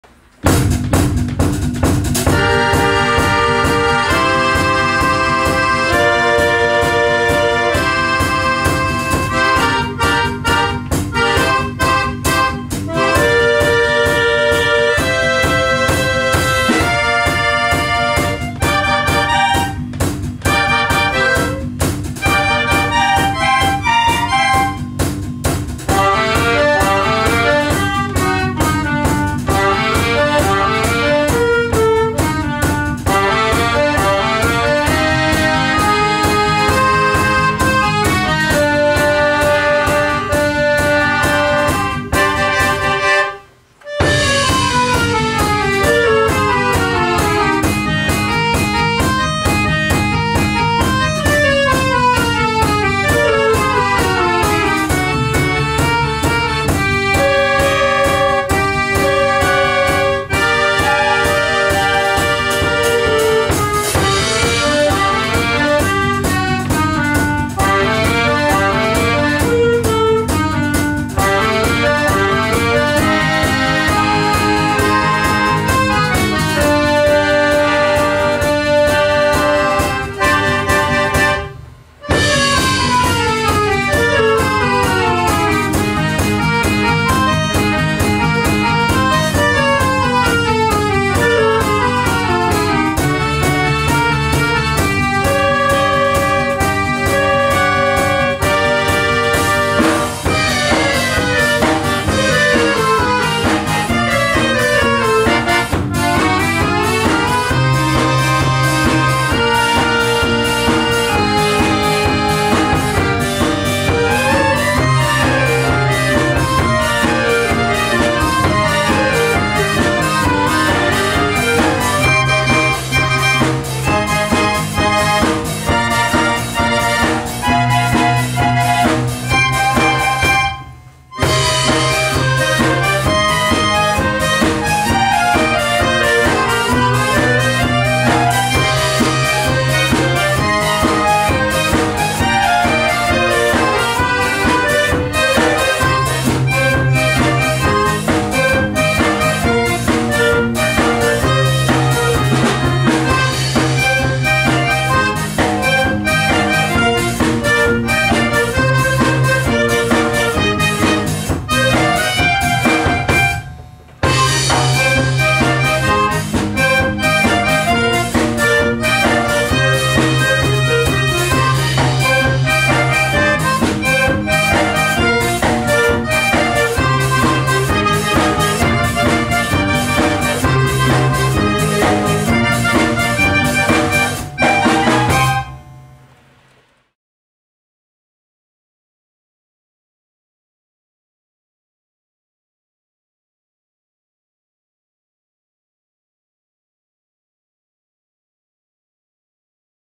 2010 Odyssée Avenir
L'Avenir accordéoniste de Châtelaine organise son concert-spectacle annuel le samedi 29 mai 2010 à la salle des fêtes du Lignon, à Genève.